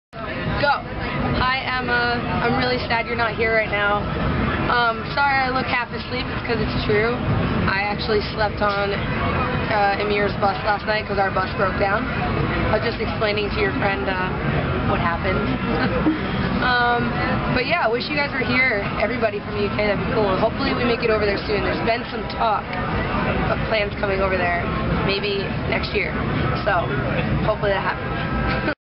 Tay Jardine Video Message
My friend shelby went to warped tour and she bumped into Taylor Jardine from we are the in crowd.
She managed to get me a video message from her.

Thank you shelby